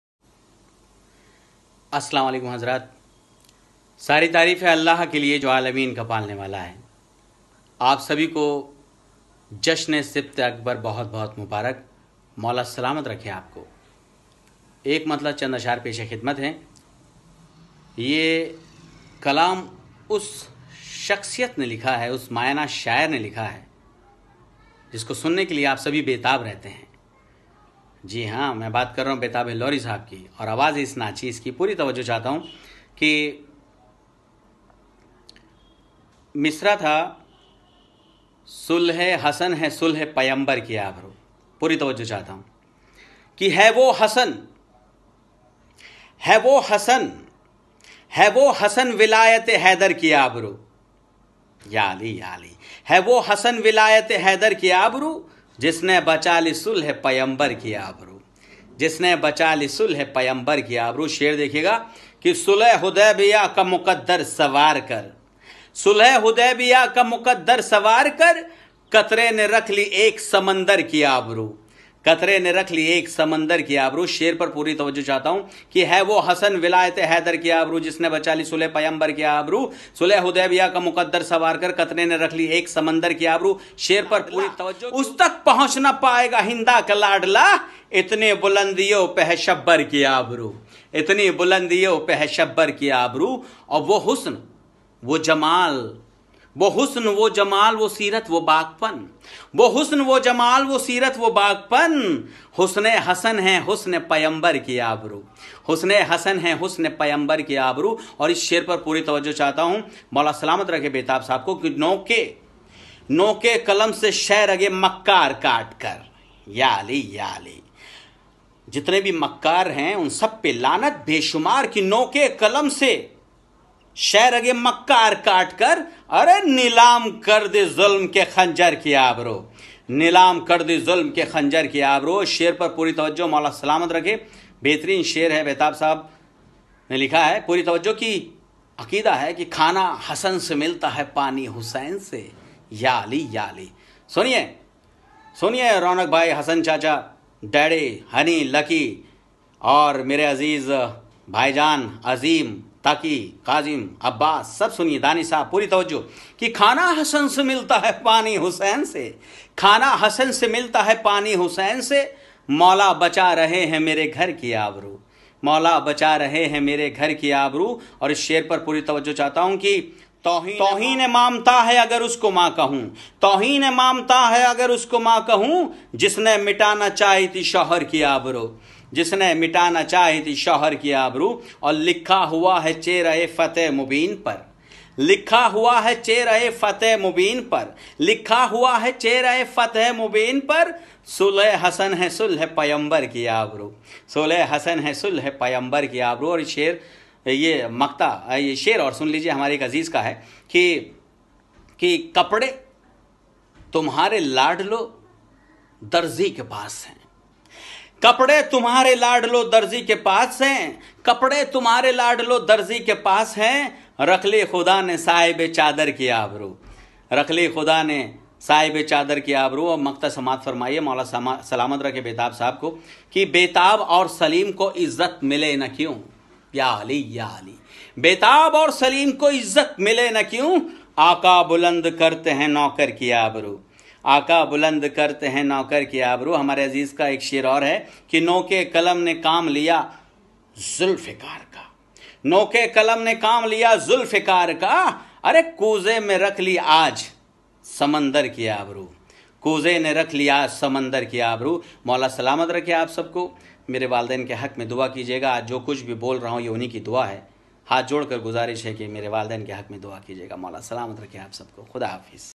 Janab Salim Zaidi (Tillu) Sb | Online Mehfil “Jashn-e-Sibt-e-Akbar” | Allahabad | 10 May 2020
Online Mehfil “Jashn-e-Sibt-e-Akbar | Ba Silsila-e-Wiladat Imam-e-Hasan (a.s), on Whatsapp Group “Zikr-e-Masoomeen” Allahabad.